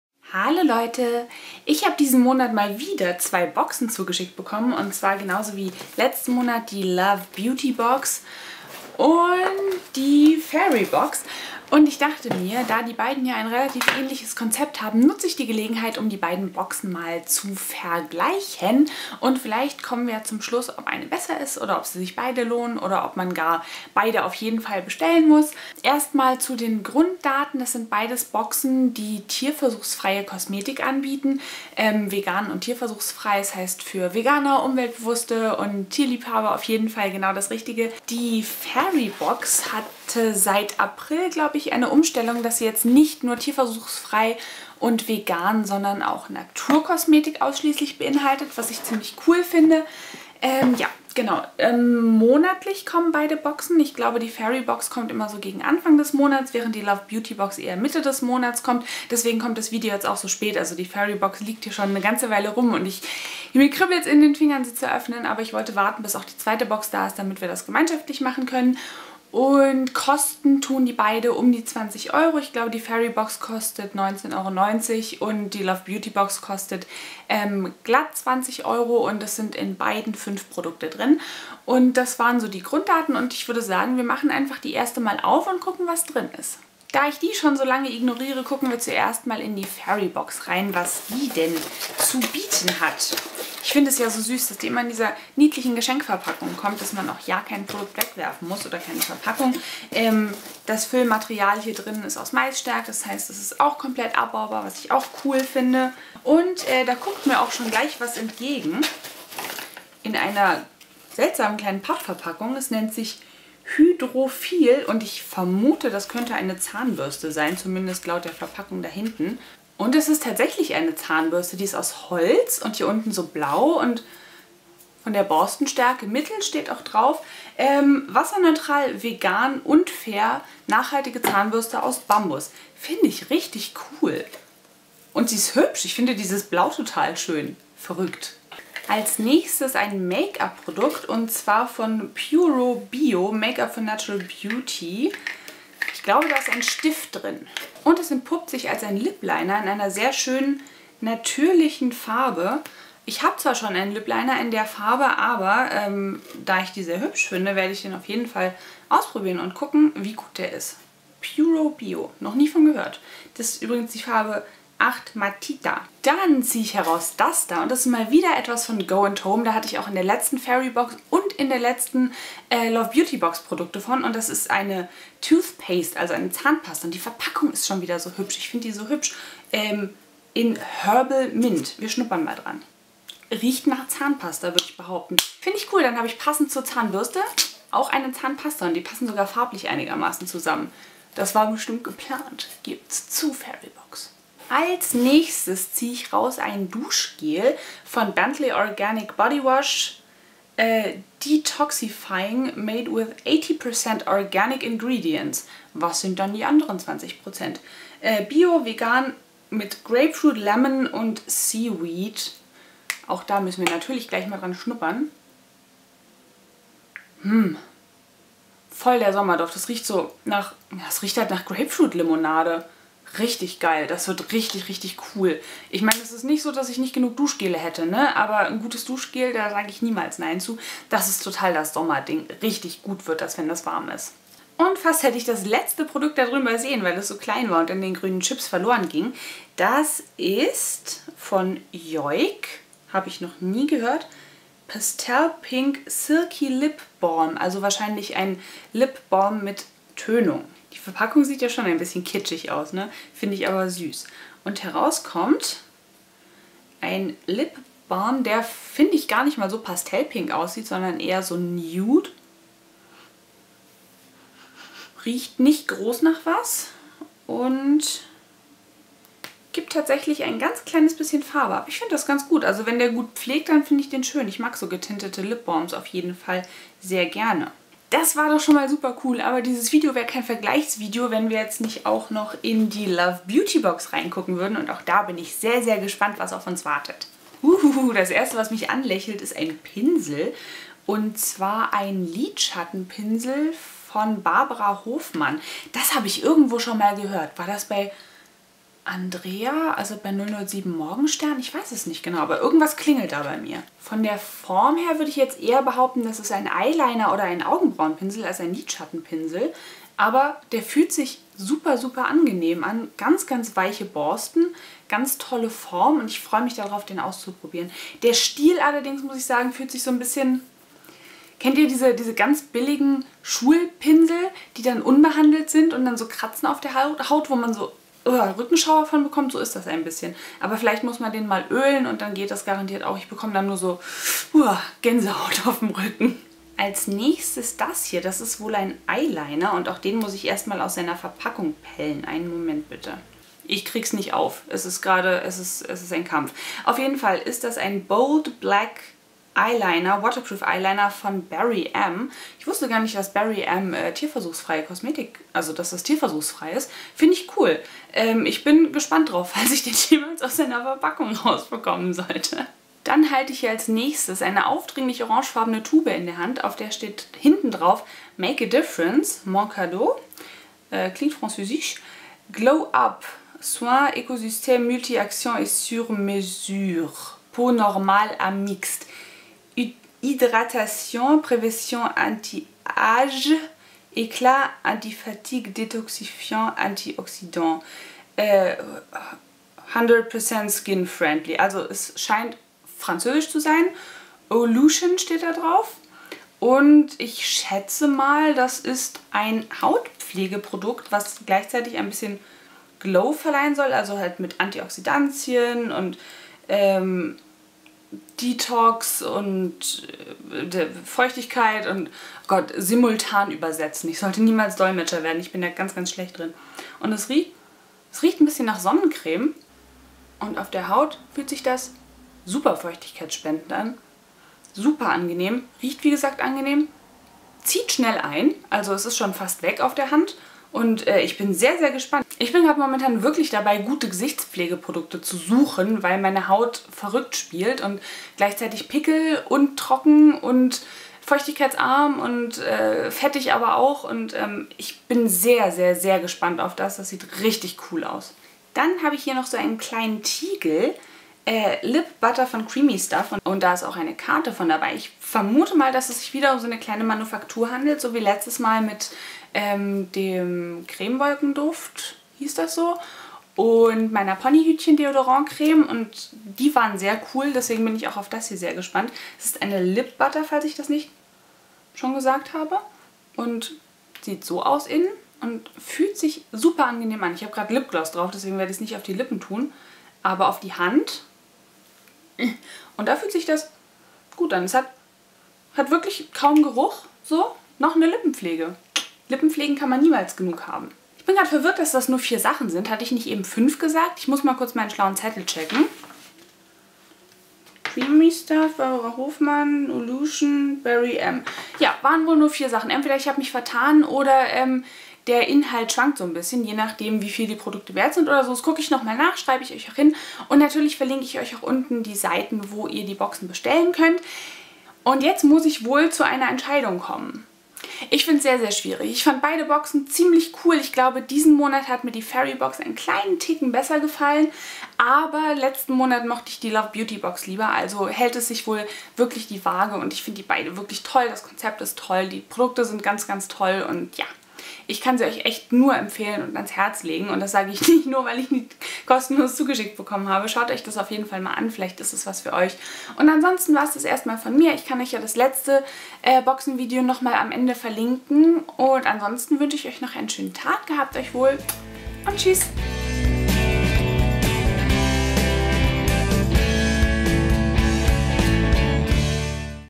FAIRY BOX vs. LOVE BEAUTY BOX
Heute vergleichen wir mal die Fairy Box mit der Love Beauty Box! Beide liefern monatlich tierversuchsfreie Kosmetik!